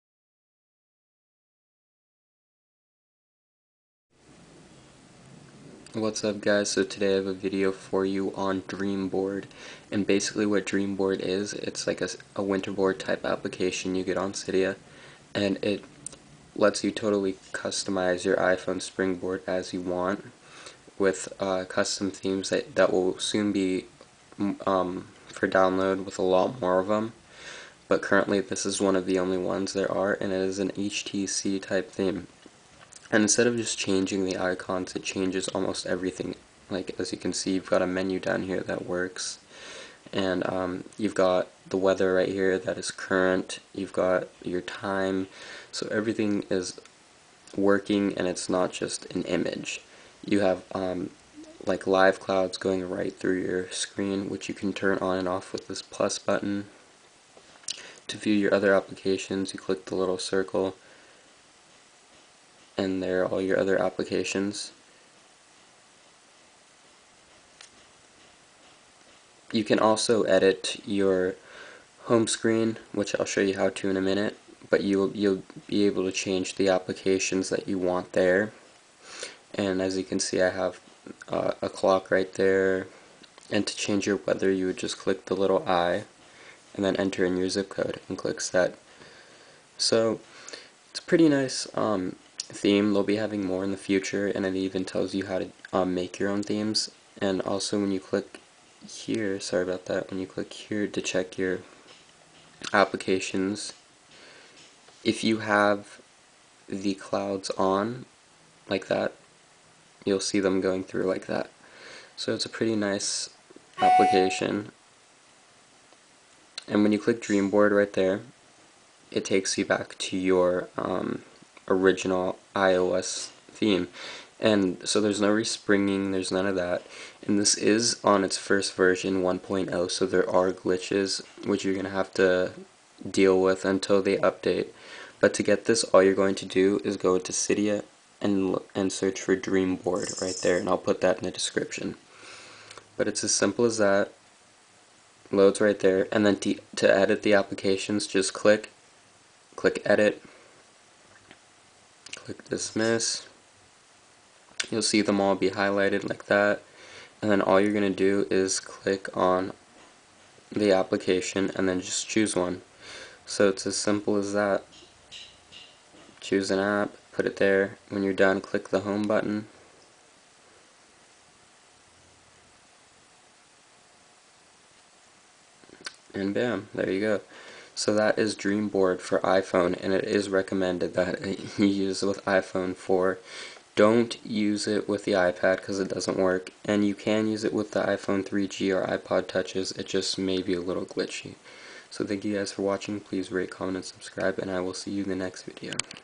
DreamBoard- "Themes at Their Best" (Tutorial)
DreamBoard is a new iDevice Theme application found on Cydia, It allows you to have the most detailed and awesome themes yet! Check out the video for more info!